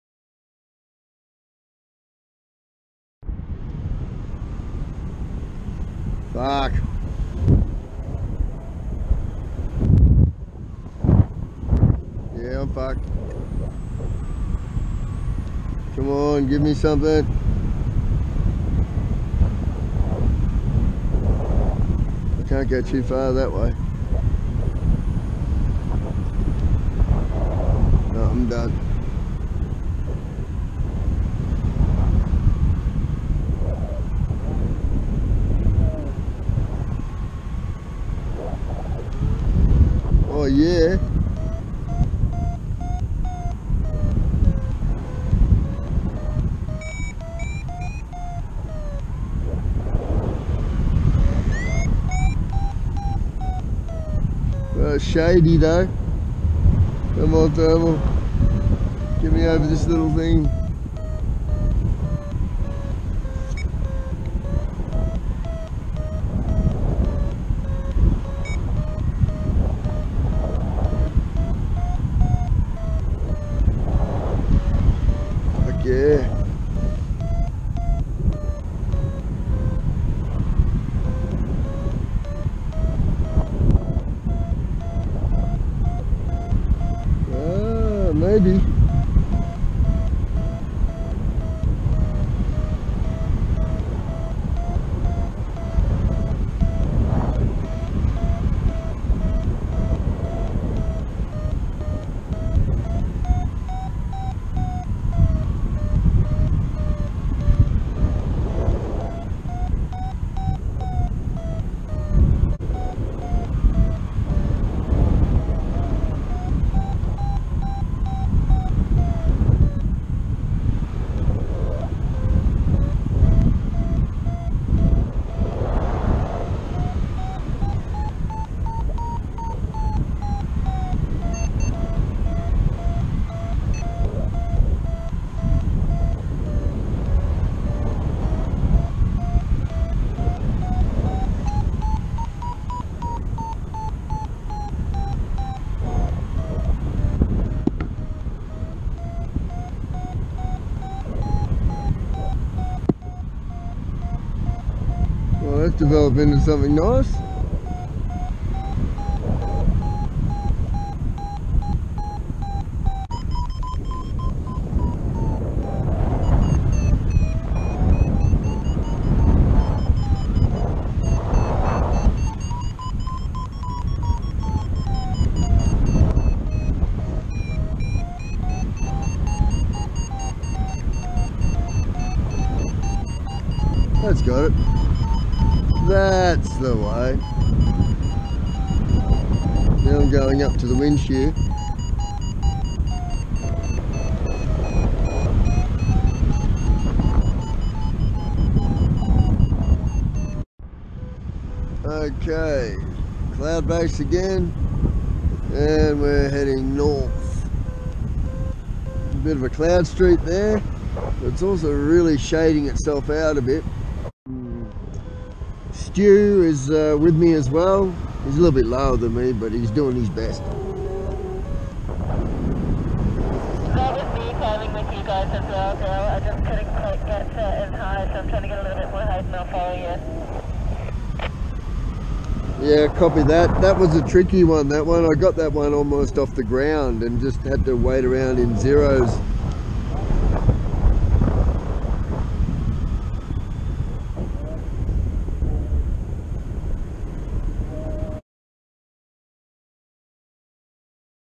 Low shaded out save
Flying Mantra7, got low just North of Barraba. The area was shaded out but managed to scratch out the remnants of a thermal.    Caution: Some Swearing